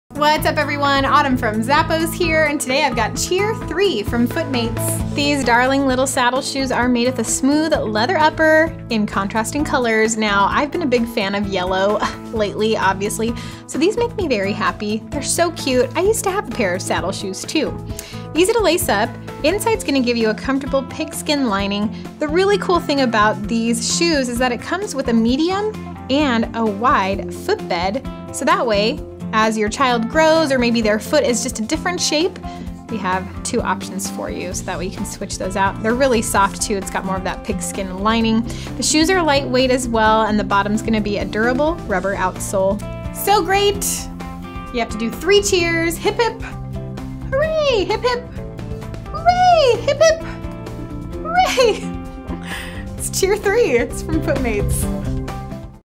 FootMates Cheer 3 (Toddler/Little Kid) SKU: 9180297
A classic and traditional saddle oxford is perfect for any occasion!
Smooth leather upper and leather saddle in contrasting colors.
Pigskin leather lining and insole.
FootMates&#174; Custom Fit System (CFS) allows you to customize the fit of the shoe to meet the individual needs of your child by including both a medium fit and a wide fit insole with each pair.
Lace-up closure provides a secure and fully adjustable fit.
Imported.

Product measurements were taken using size 8.5 Toddler, width M/W. Please note that measurements may vary by size.